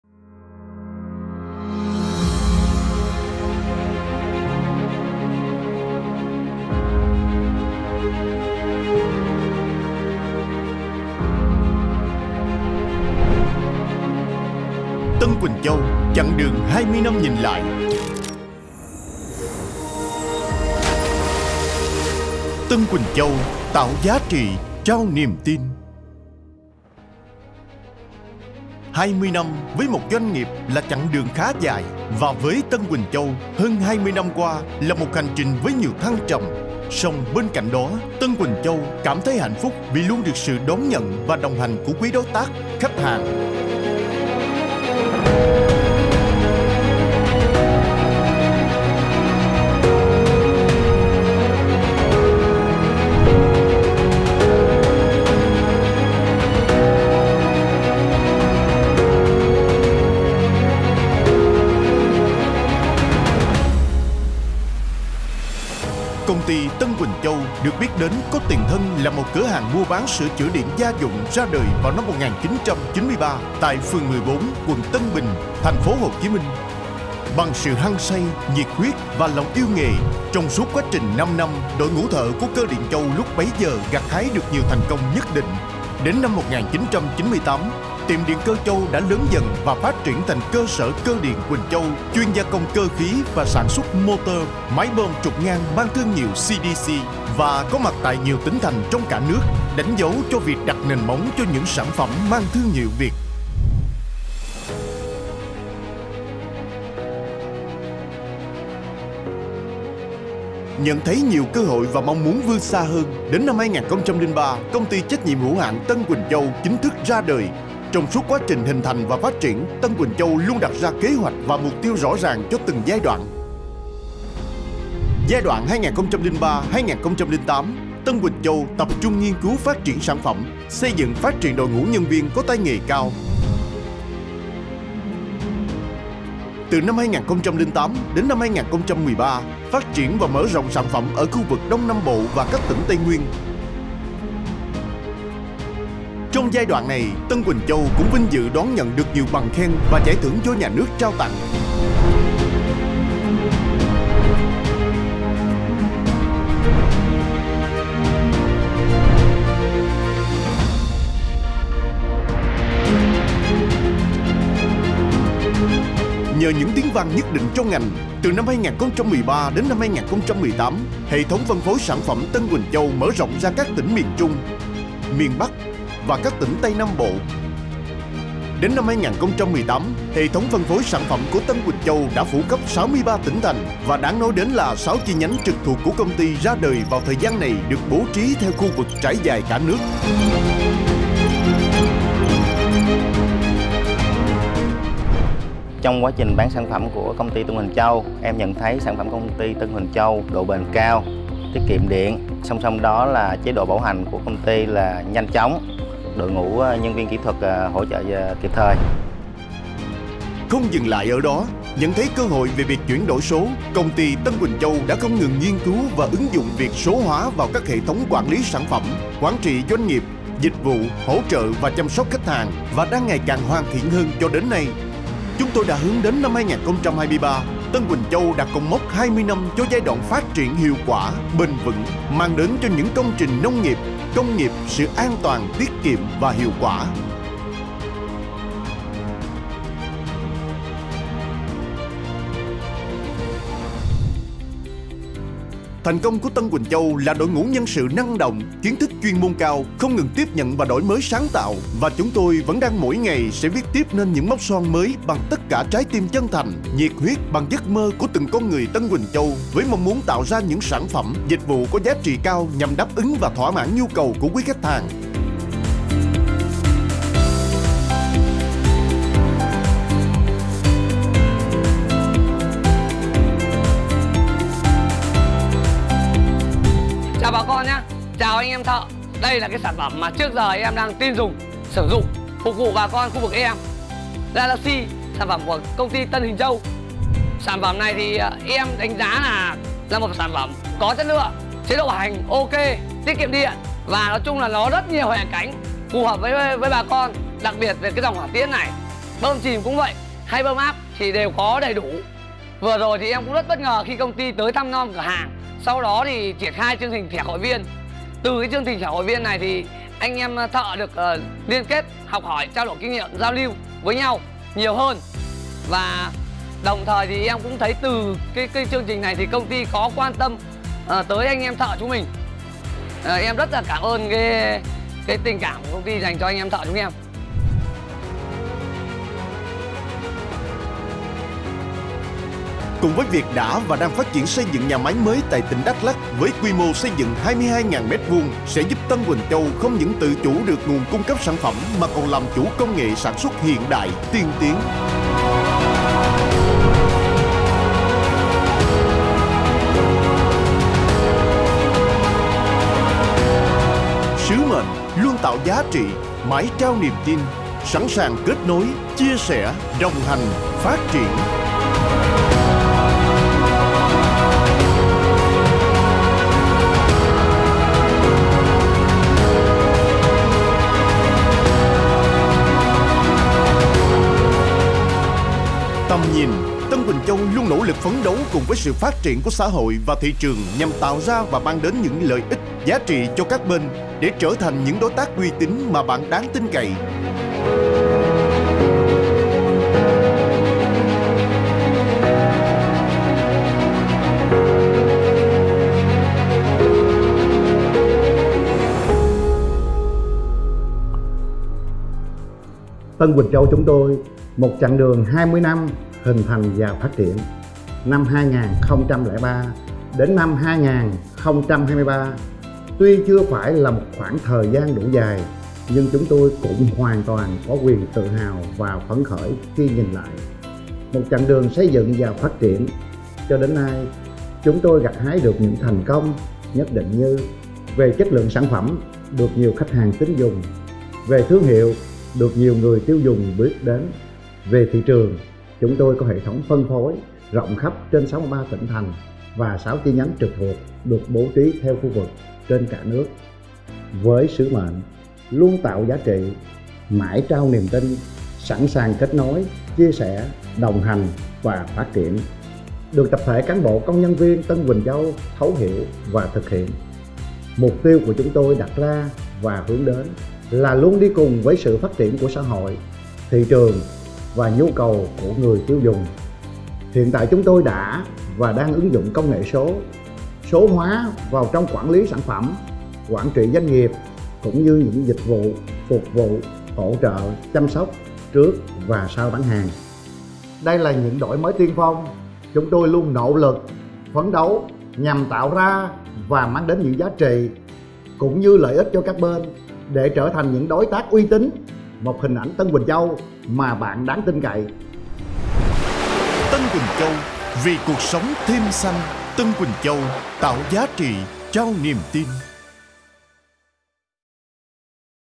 Tân Huỳnh Châu chặng đường 20 năm hình thành và phát triển
🌟 Kỷ niệm 20 năm. Nhìn lại hành trình  vời cùng Công ty Tân Huỳnh Châu! 🌟
Trong video đặc biệt này, chúng tôi rất vui chia sẻ với quý đối tác, khách hàng những khoảnh khắc quan trọng nhất trong lịch sử hình thành và phát triển của công ty. Từ những ngày đầu tiên khởi nghiệp, qua những thách thức và thành công, đến việc trở thành một trong những công ty tiên phong trong lĩnh vực máy bơm nước, máy bơm điện chìm, hoả tiễn có mặt khắp 63 tỉnh thành trên cả nước.
CÔNG TY TNHH TÂN HUỲNH CHÂU
📍 Địa chỉ: 31B Nguyễn Văn Bứa, Ấp 5, Xuân Thới Sơn, Hóc Môn, TP.HCM